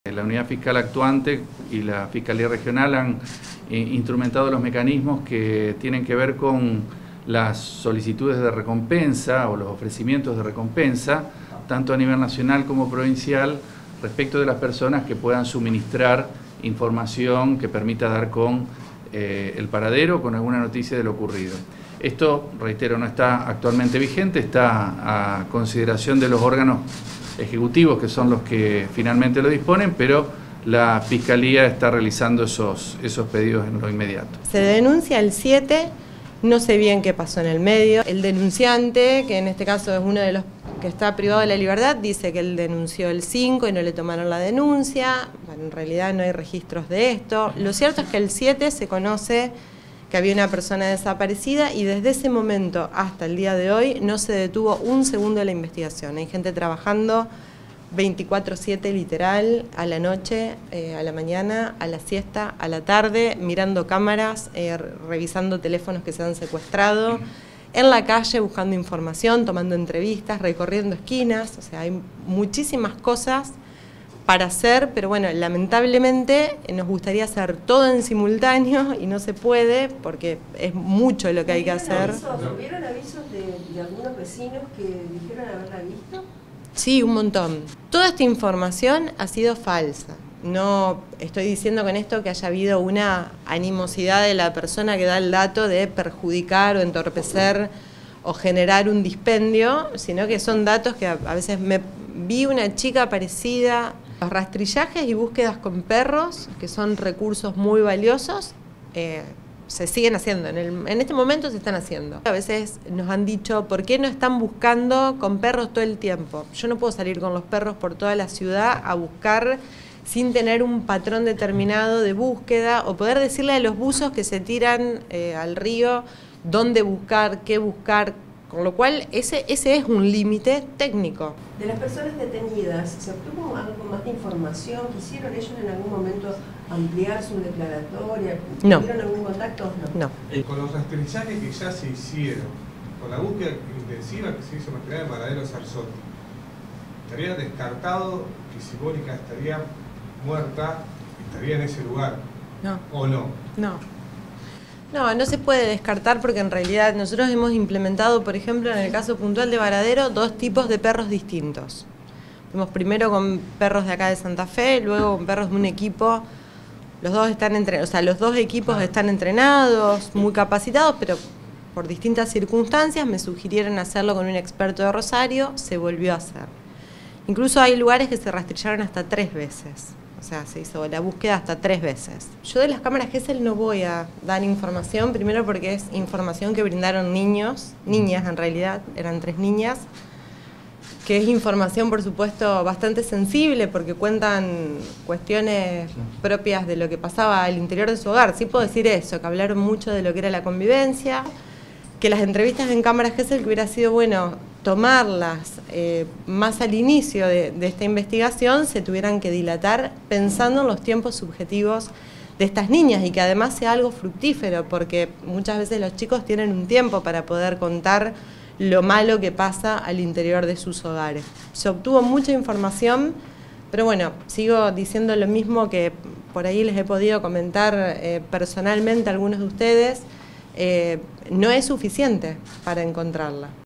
CONTINÚA LA BÚSQUEDA DE MÓNICA AQUINO.
JORGE NESSIER – FISCAL REGIONAL.
ALEJANDRA DEL RÍO AYALA – FISCAL MPA